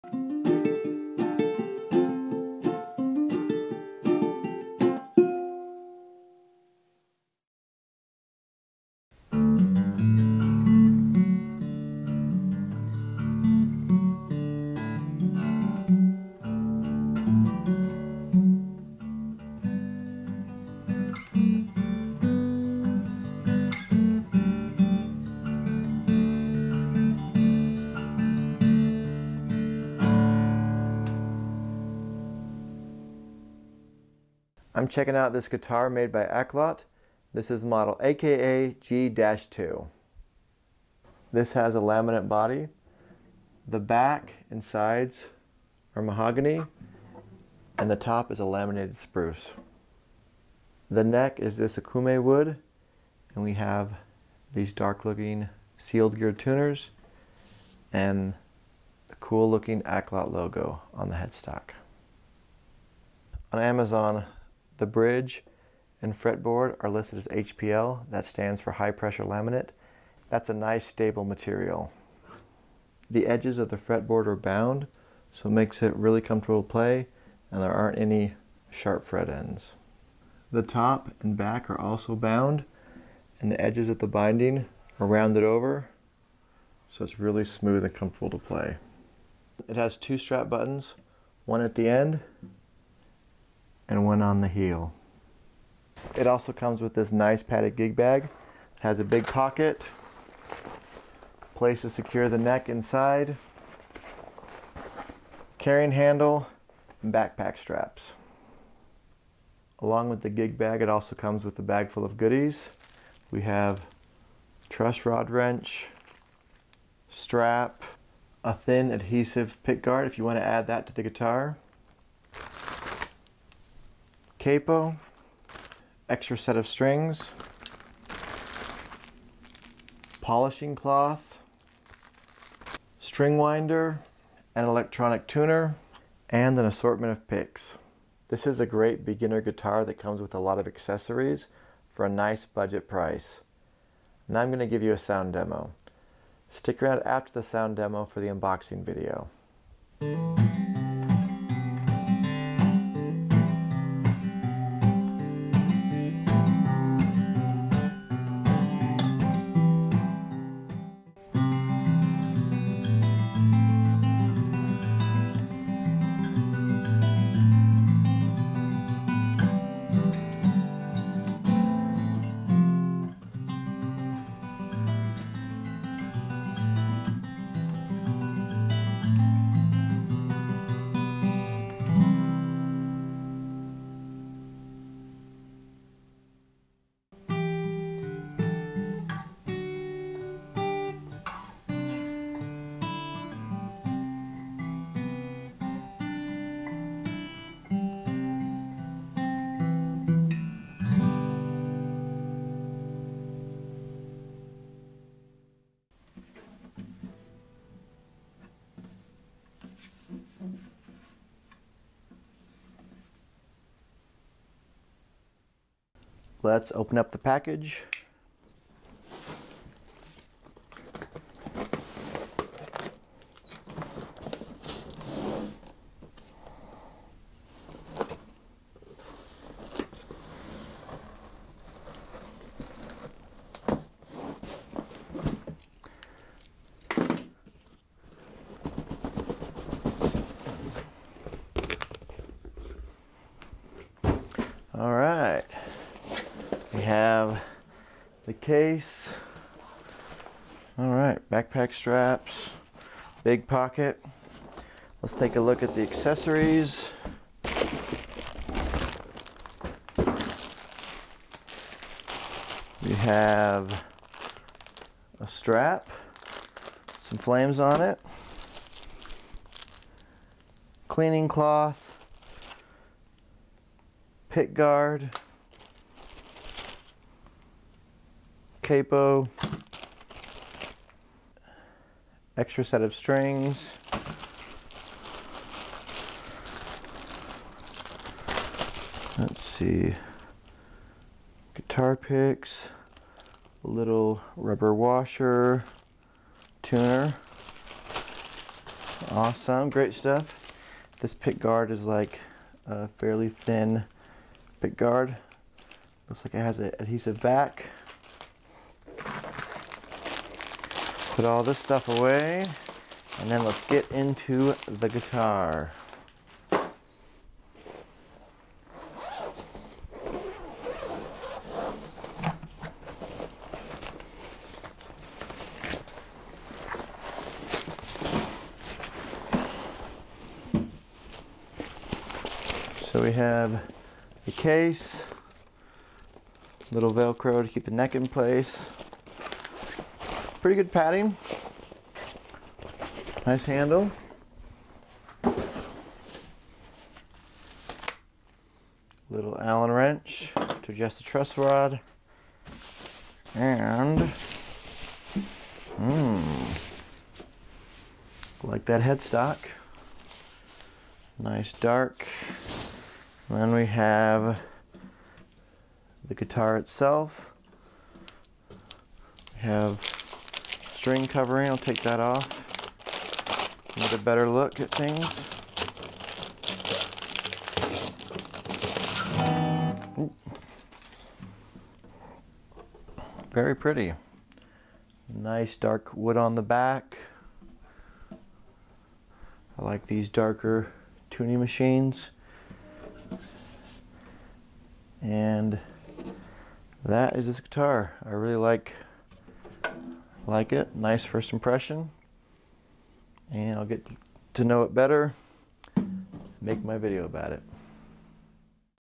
Aklot Acoustic Guitar
Checking out this newly revamped Aklot acoustic guitar.

Amazon US
Amazon CA
Amazon JP：
Amazon UK:
Amazon FR
Amazon IT
Amazon ES
Amazon DE